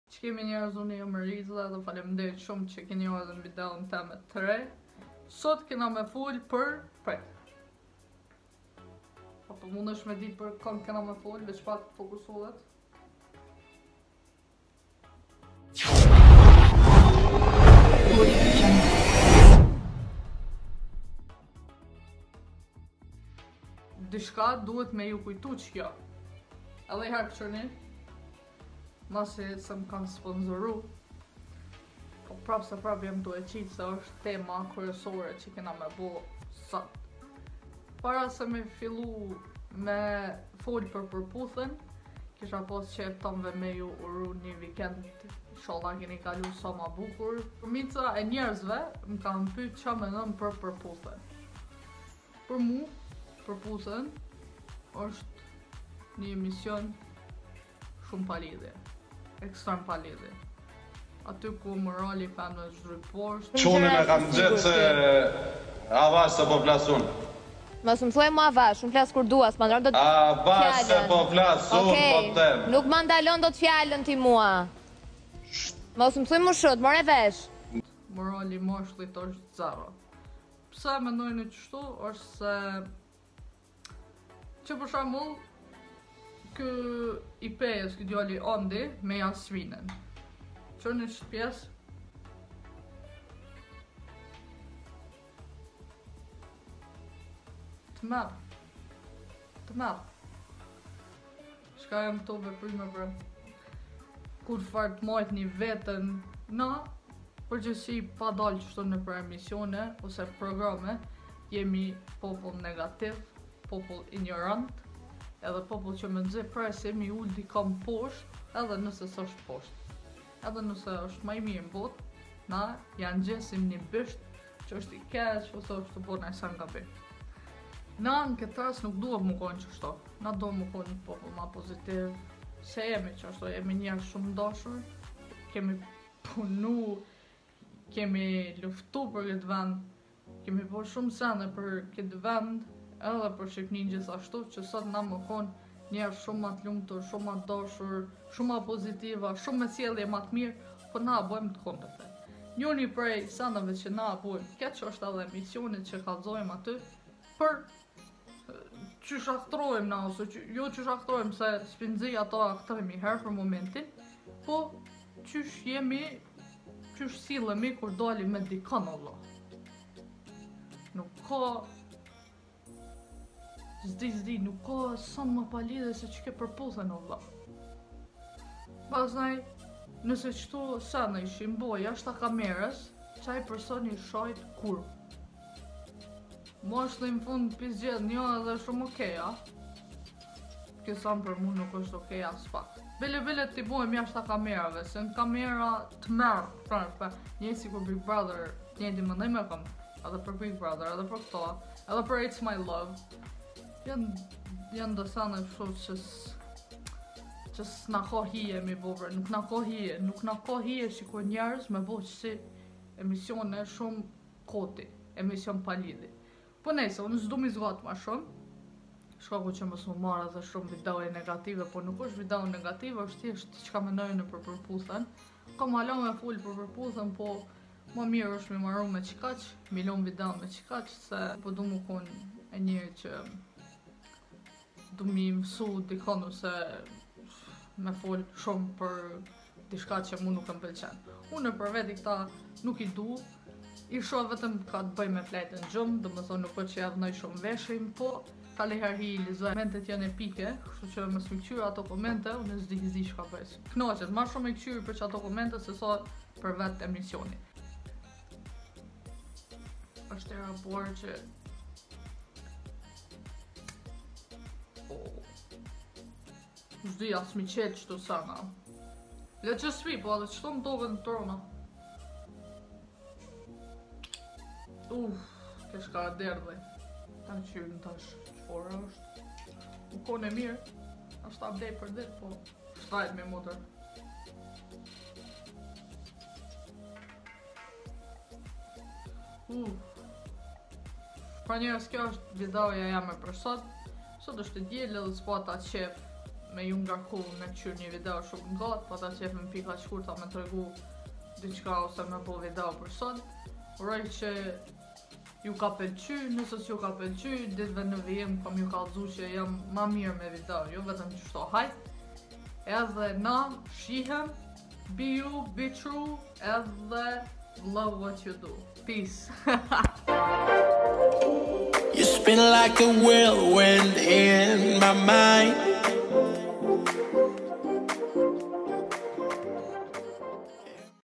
Mendimi per programin Përputhen
Ky o mendimi im personal. Kush preket a dicka nuk mbaj pergjegjesi. Puq